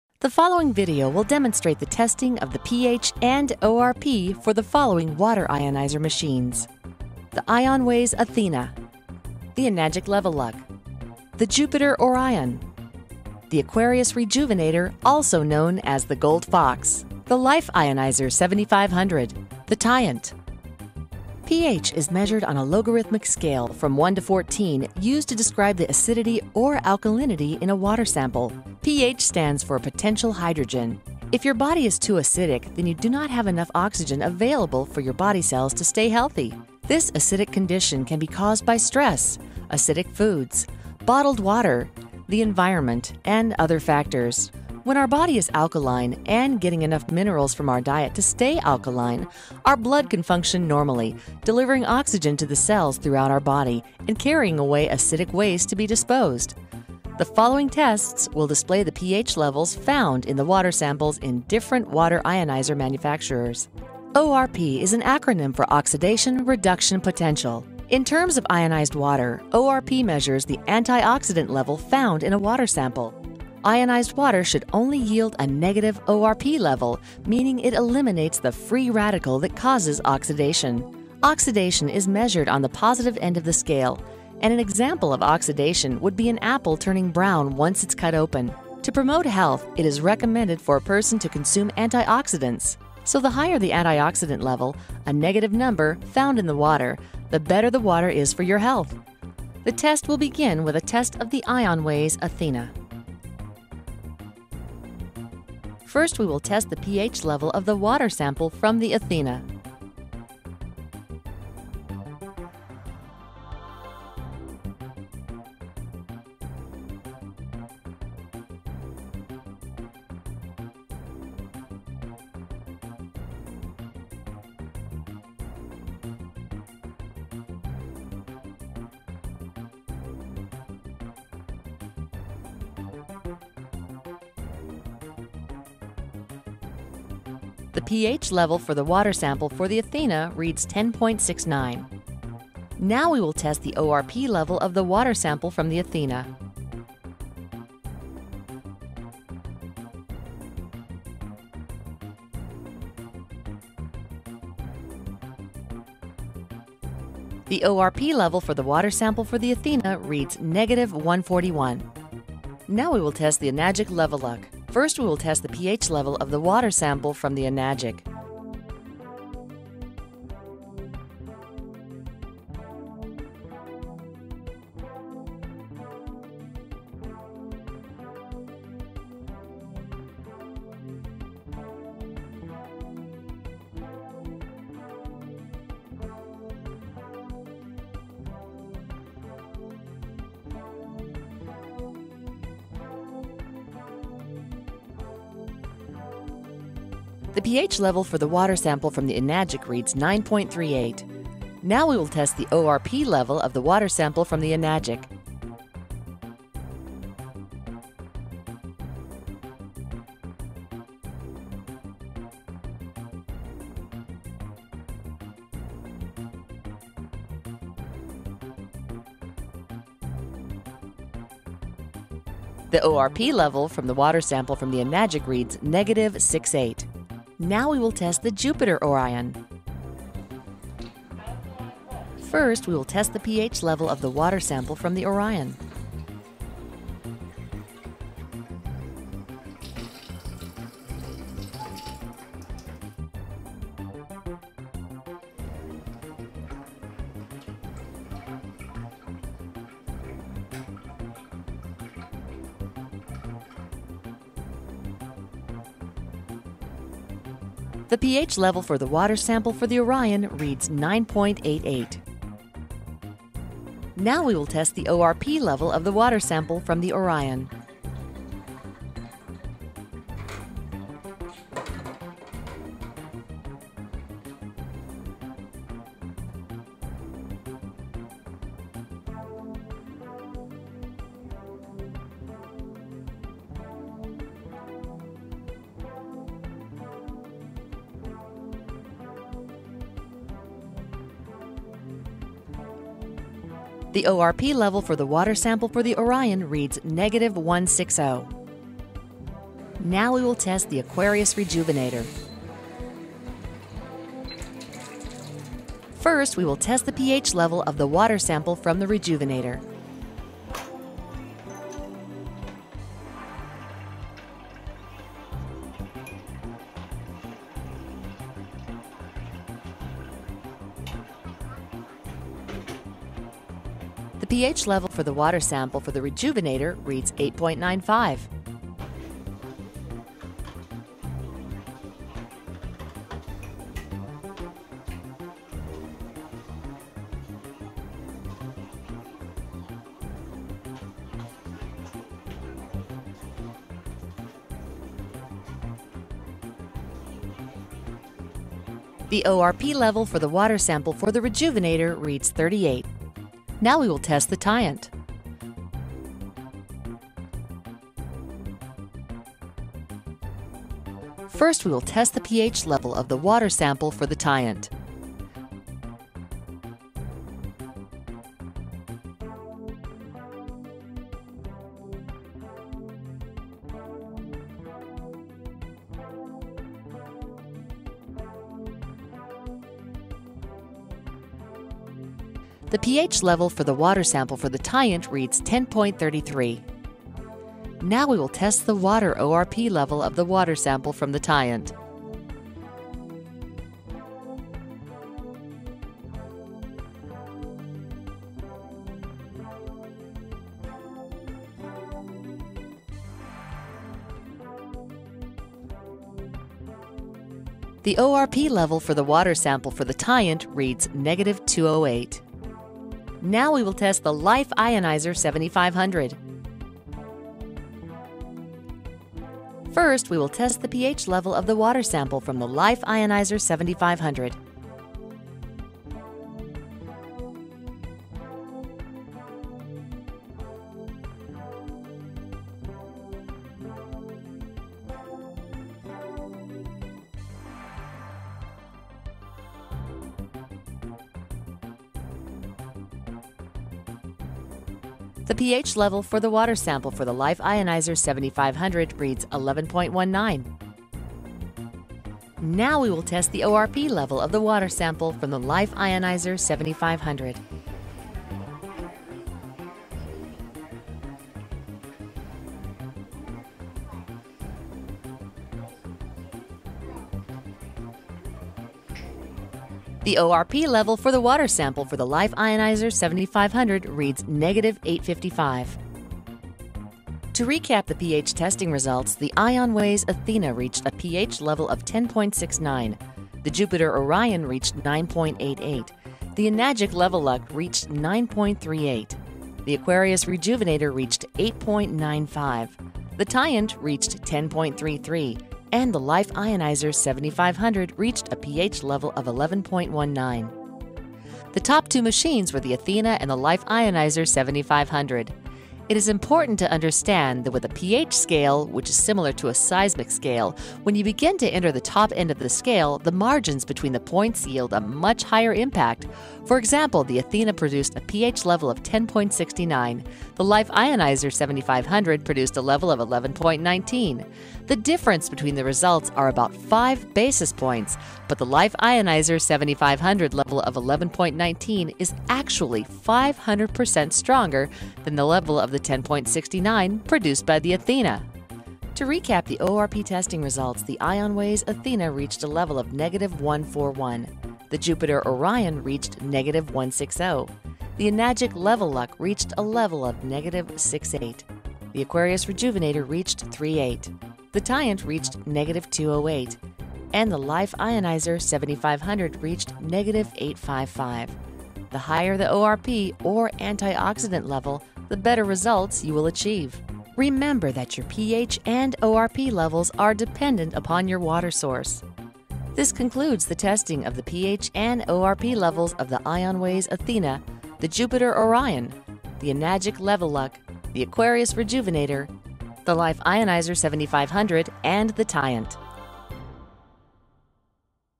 Alkaline Ionizer Water Test
Check out these pH and ORP ionizer water tests from five different ionizer machines: Jupiter Orion, Ion Way Athena, Life Ionizer 7500, Tyent, Aquarius Rejuventaor Gold Fox and the Eganic LeveLuk. See which machines measured the highest pH and ORP readings, so you have a more information to help you decide which one to purchase.